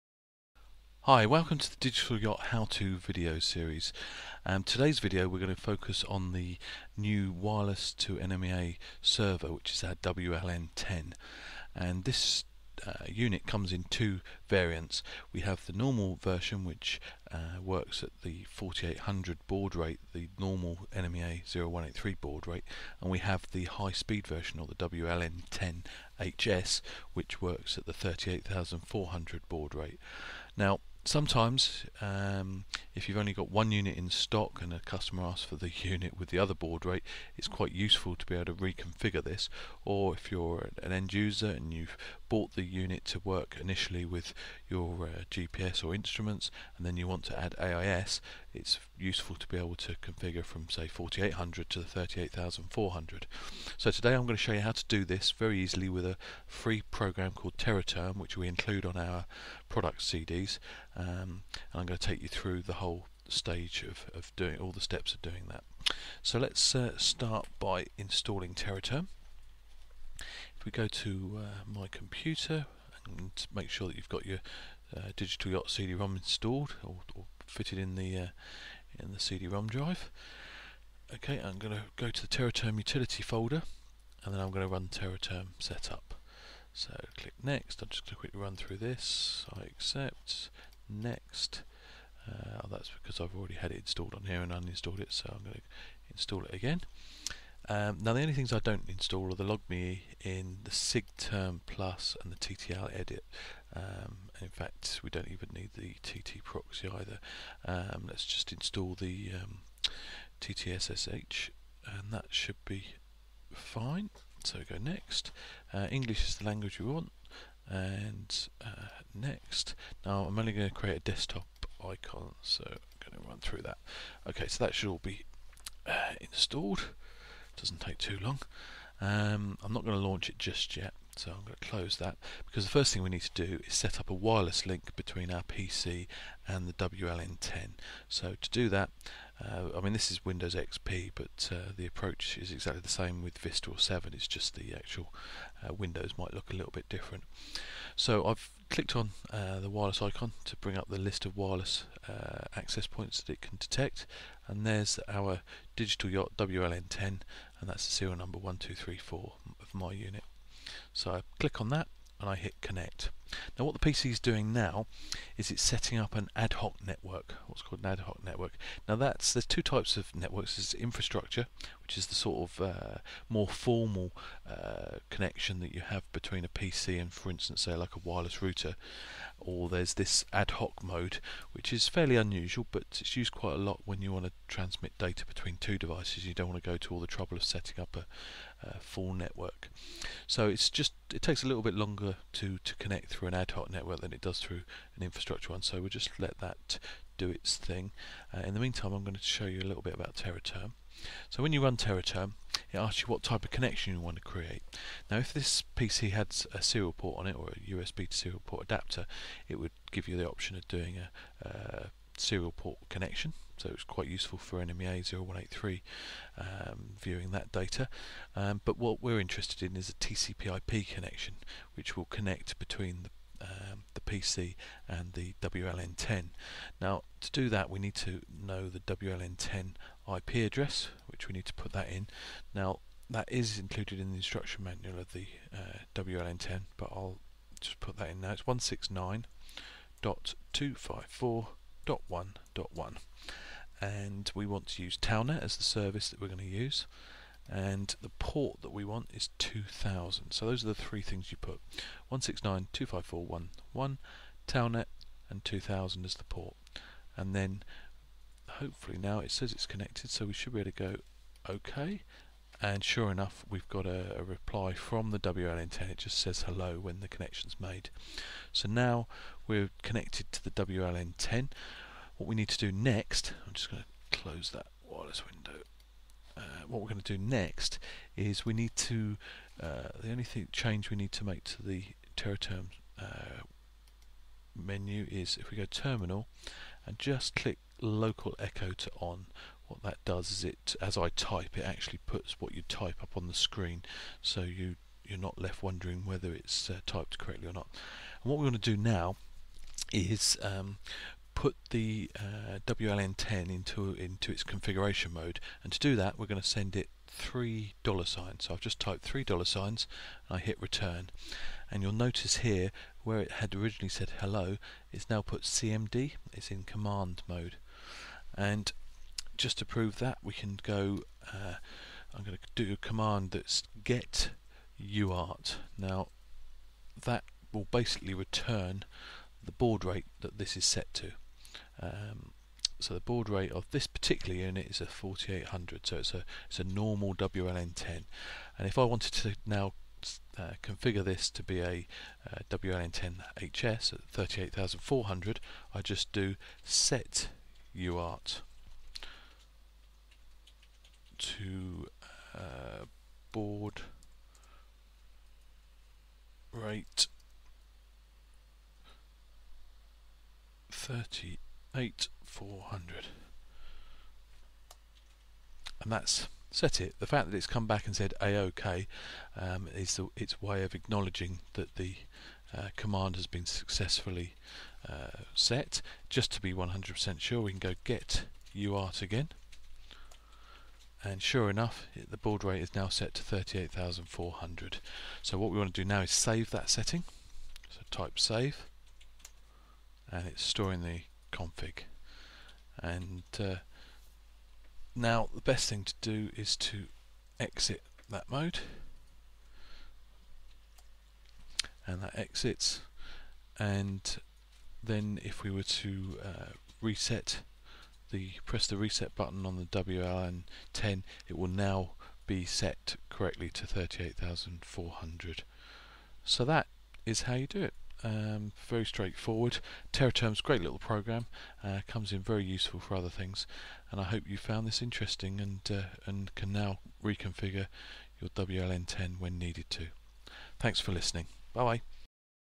Configuring the WLN10 with TeraTerm 2 - No. 5 Technical Video
Chief Technology Officer, Paul Sumpner, takes you through the very simple steps to help you configure the baud rate for the WLN10. It's extremely easy and an incredibly simple with the help of this 7 minute video.